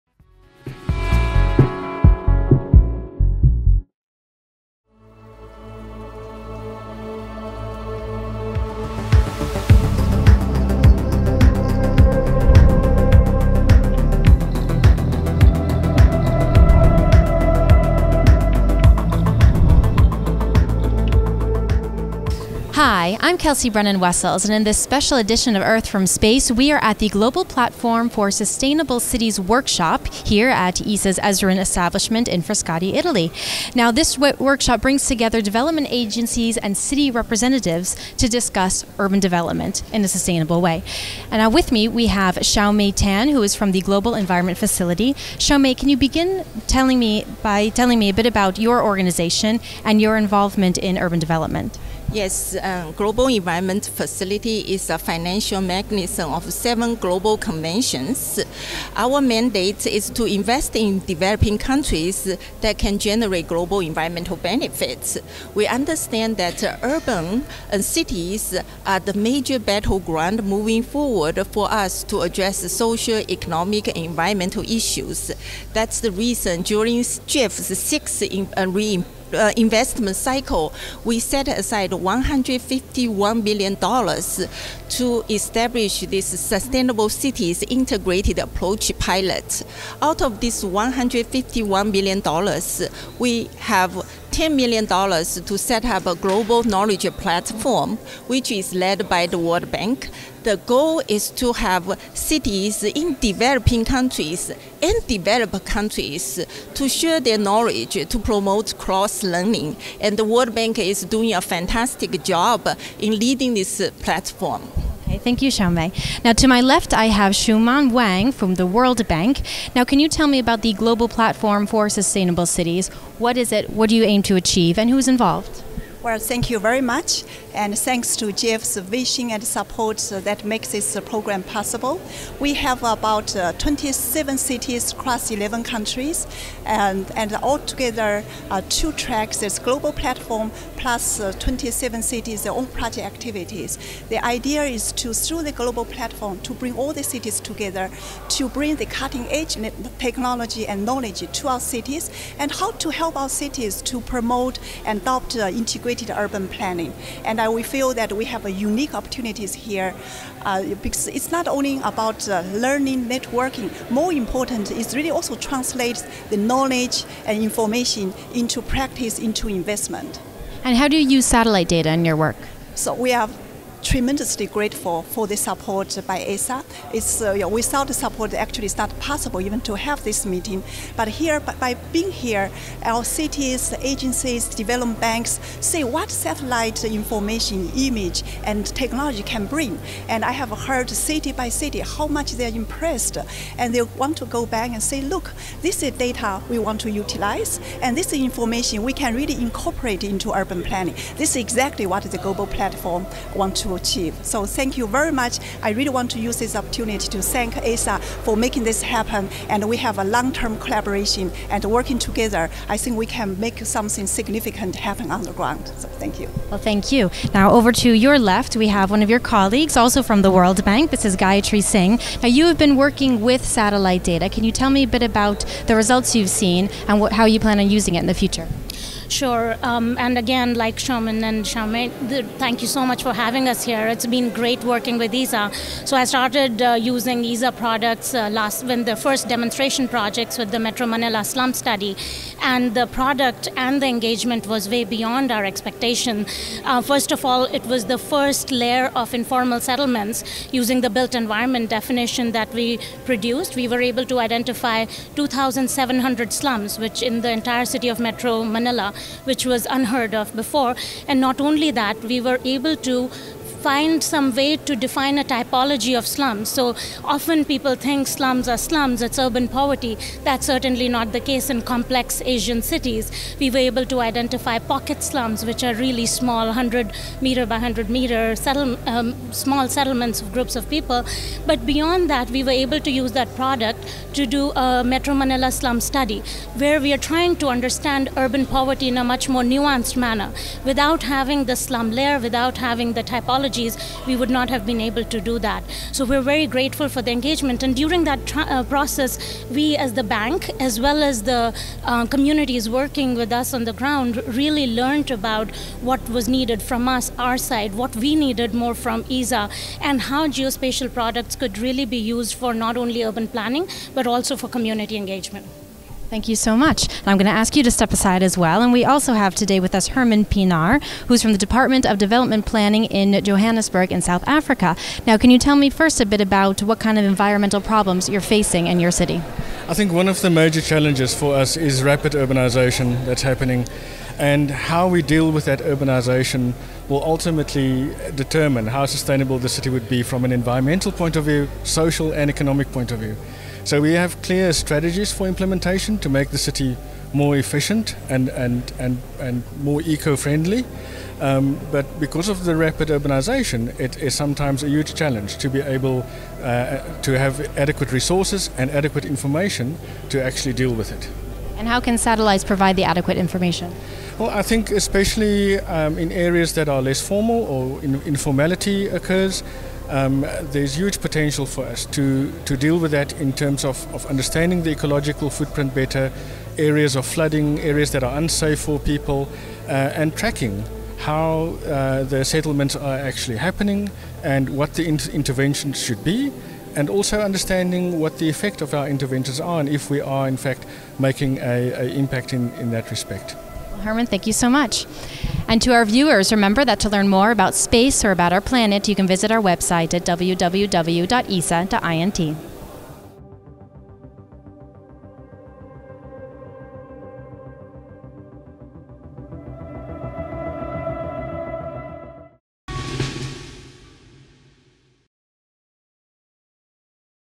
Earth from Space: Special edition
Discover more about our planet with the Earth from Space video programme. During the Global Platform for Sustainable Cities working group meeting at ESA’s ESRIN establishment in Italy, four experts join the show to discuss how satellite data can be used to promote sustainable cities.

The four guests are Xiaomei Tan: Senior Climate Change Specialist, Global Environment Facility; Xueman Wang: Global Platform for Sustainable Cities Coordinator, World Bank; Gayatri Singh: Urban Development Specialist, World Bank City Planning Labs Initiative; Herman Pienaar: Director of City Transformation and Spatial Planning, City of Johannesburg.